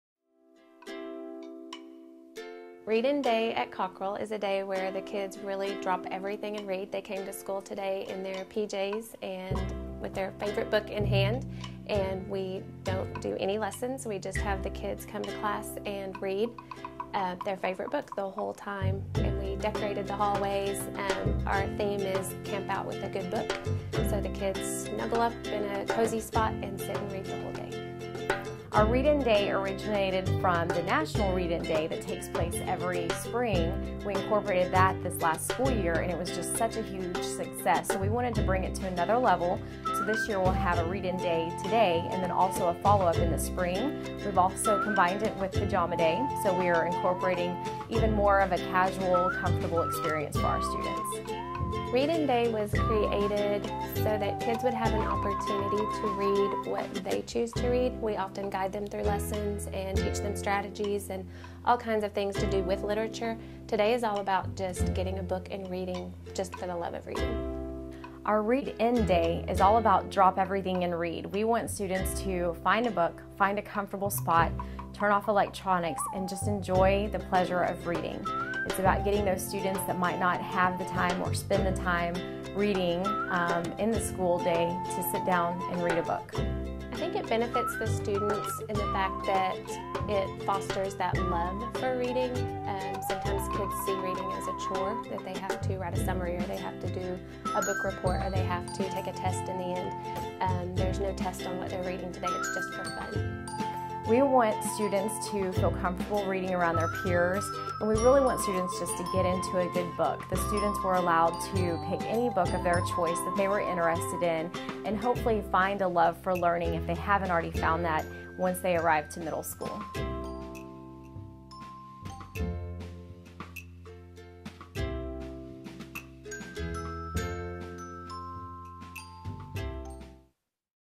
"Read-In Day" - Cockrill Middle School
Judy Mercer and Jenny Zornes comment on this day dedicated to reading in their school.
October 2015